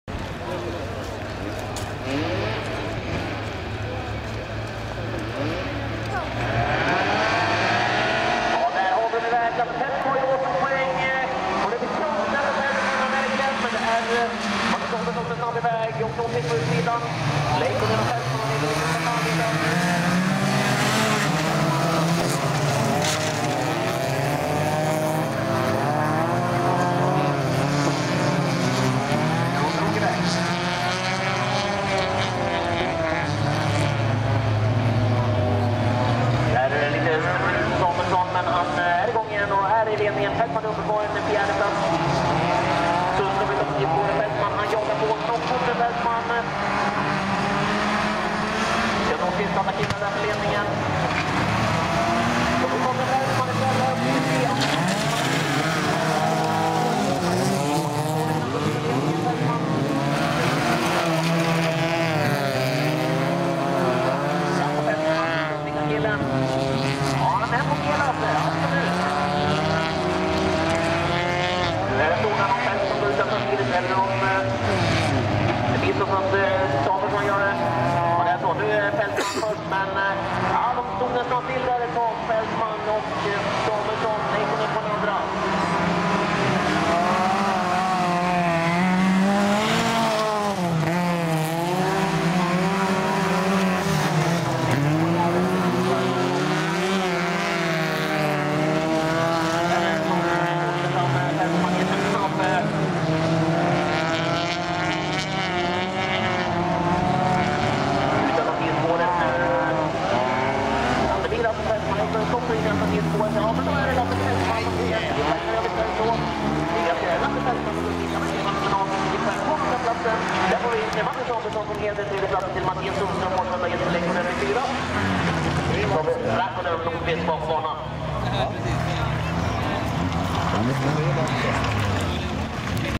Gestrikefestivalen 10-års jubileét maj 2016 Storvik
Folkrace Gestrikefestivalen_Storvik 20160529
Team Sirpa Racing SMK Hedemora 131 Jesper Lehikoinen